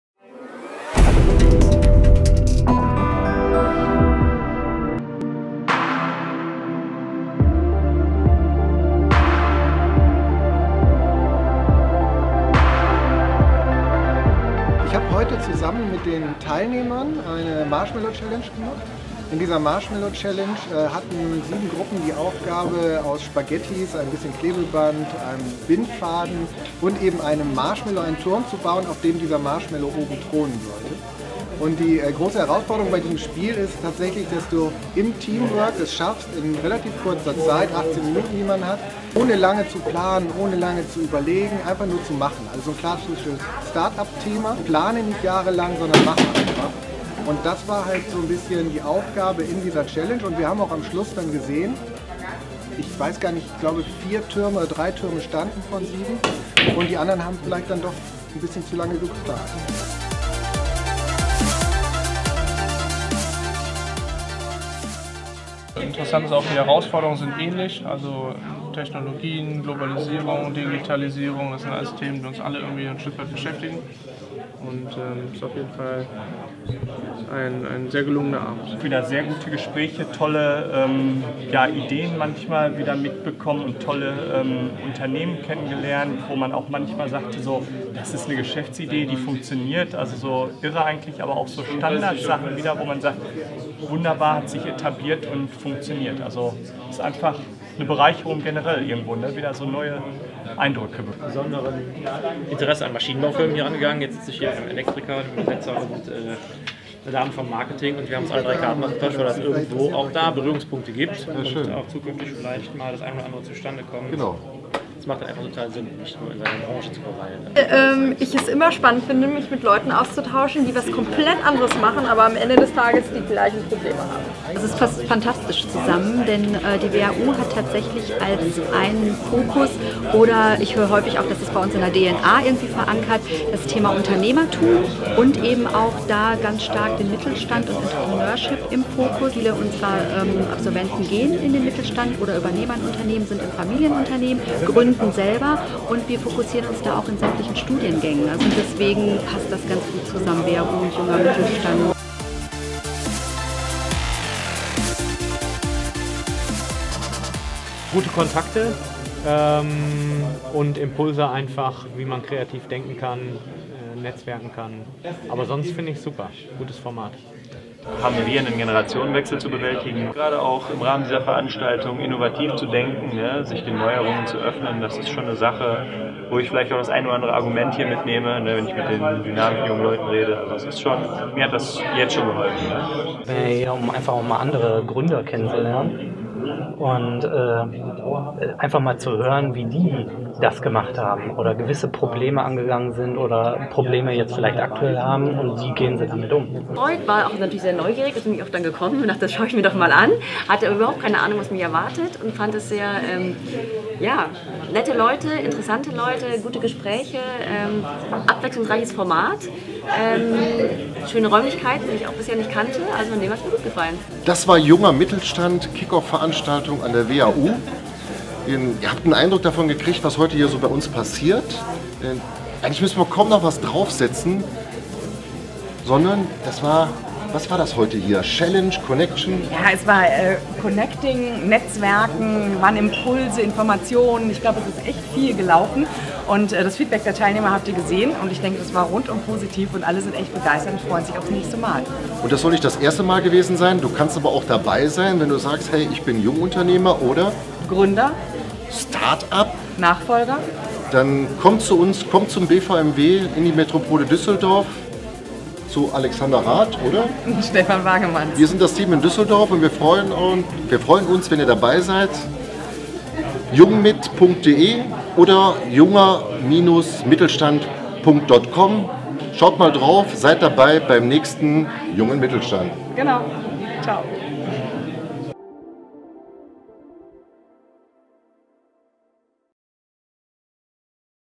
BVMW JUNGER MITTELSTAND Kick-Off 07. November 2017
Beim Kick-Off BVMW JUNGER MITTELSTAND trafen sich am 07. November 2017 Gründer, Start-Ups und Nachfolger zum Connecten und "Querdenken". In den Räumen der WHU - Otto Beisheim School of Management in Düsseldorf wurde erst genetztwerkt und danach zeigten die Teilnehmer echten Teamgeist bei der Marshmallow-Challenge, die durch Jørn Rings von der NEU - Gesellschaft für Innovation angeleitet wurde.
Video: Ingo Musial - 20.7 Media